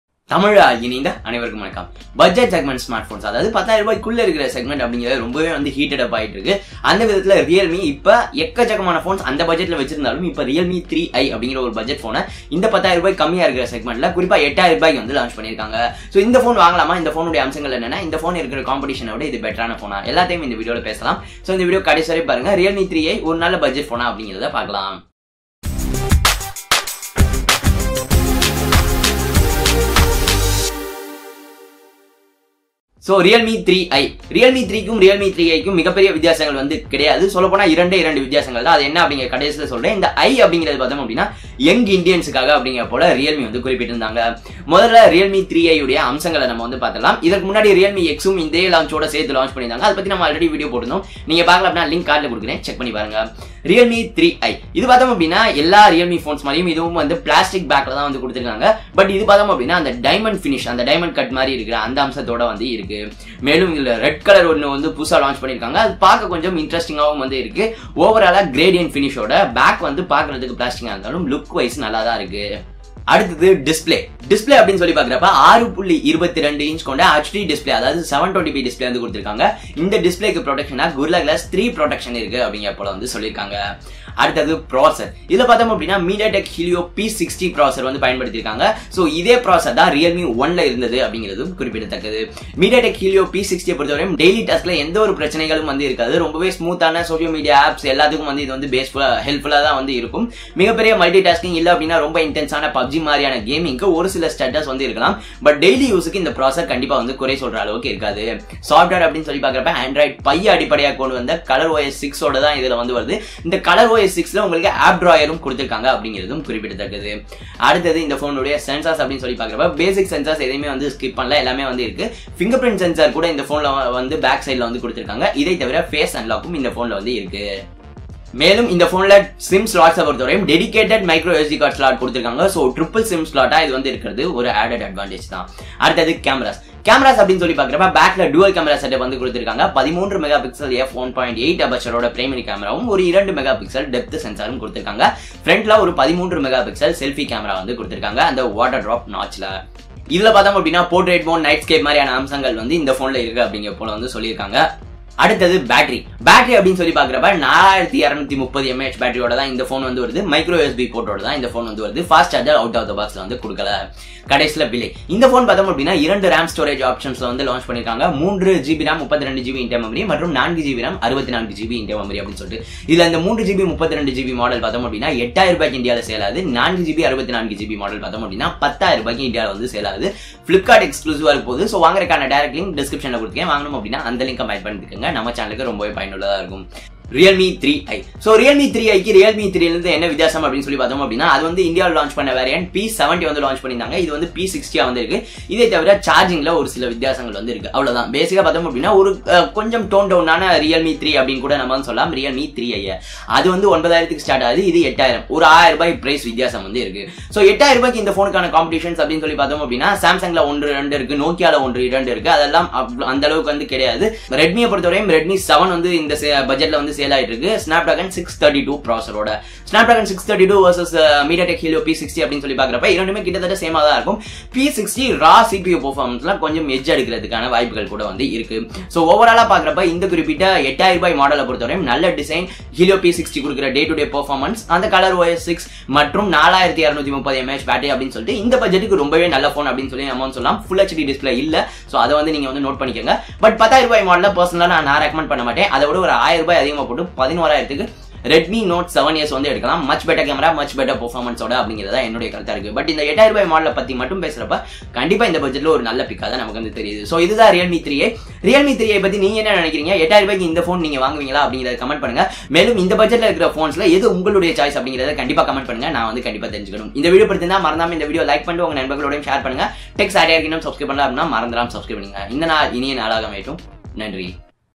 8000 ரூபாய்க்கு இவ்ளோ நல்ல போனா? உண்மையாவே வாங்கலாமா? Realme 3i - My Opinions in Tamil | Tech Satire
Realme 3i in Tamil - Not a Unboxing / Review ! Specs & My Opinions! Same asss Realme 3? | Tech Satire